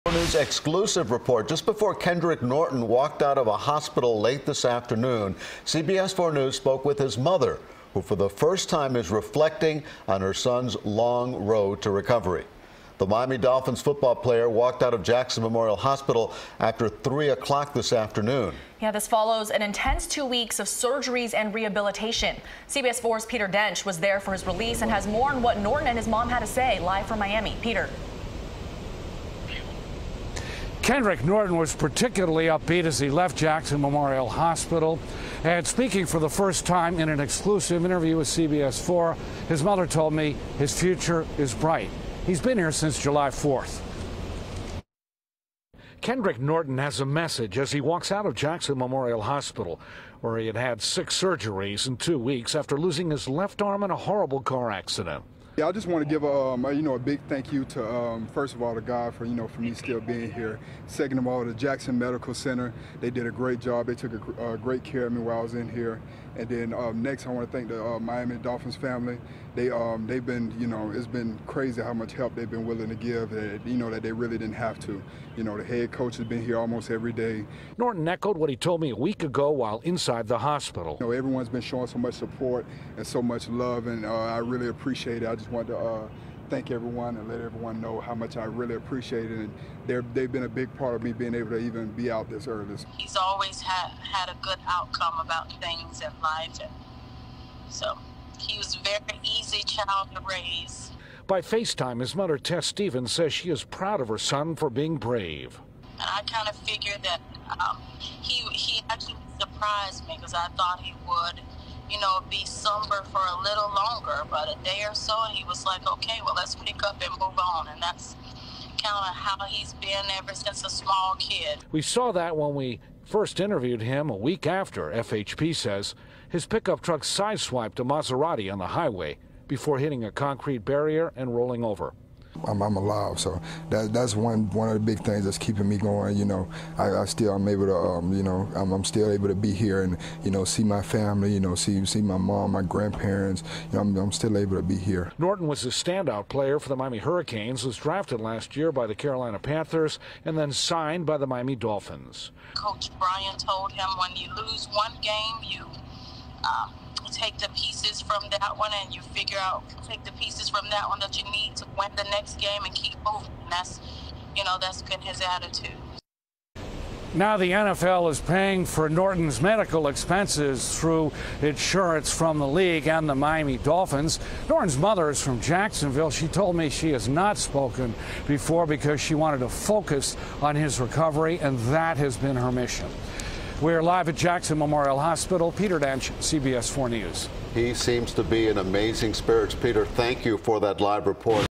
Miami Dolphins DT Kendrick Norton Released From Hospital, Mother Reflects On His Road To Recovery
Miami Dolphins defensive tackle Kendrick Norton, was released from the hospital Thursday exactly two weeks after being rushed into emergency surgery after suffering multiple injuries in a car crash that required his left arm to be amputated.